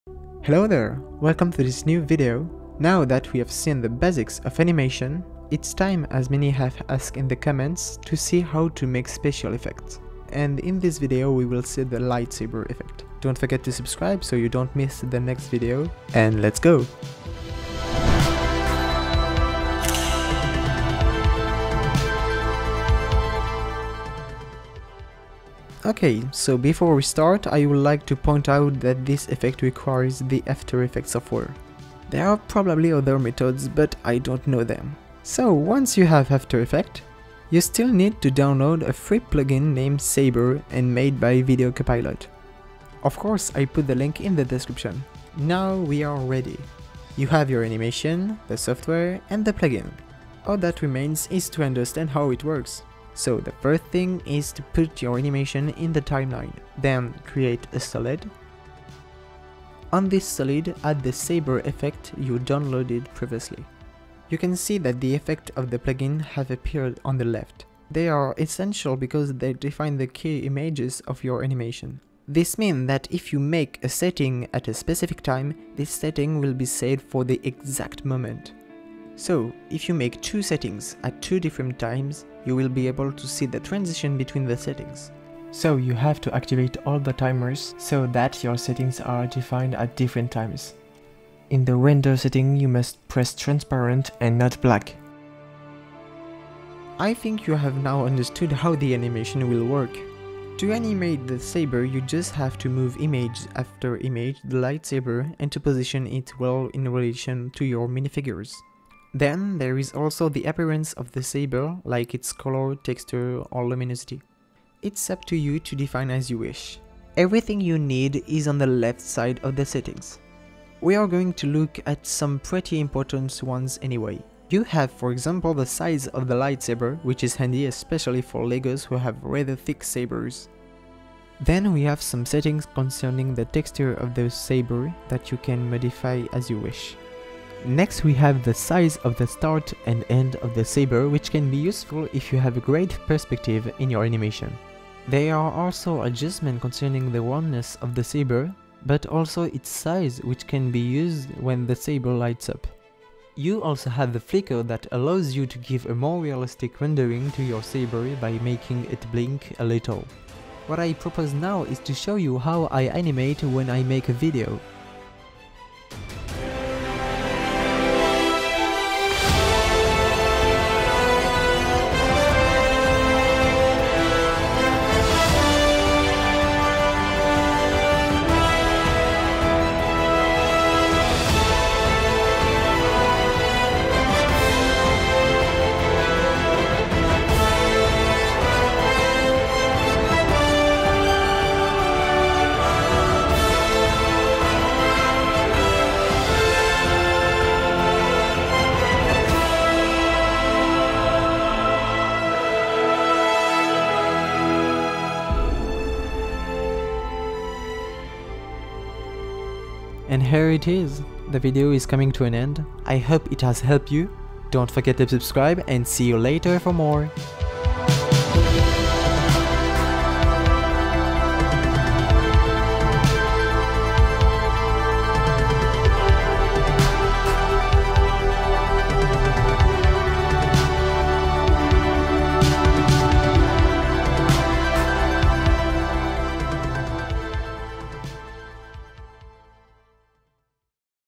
How to make the lightsaber effect (After effect tutorial)
I hope you enjoyed this video. If you want more videos like this don't hesitate to ask me in the comments.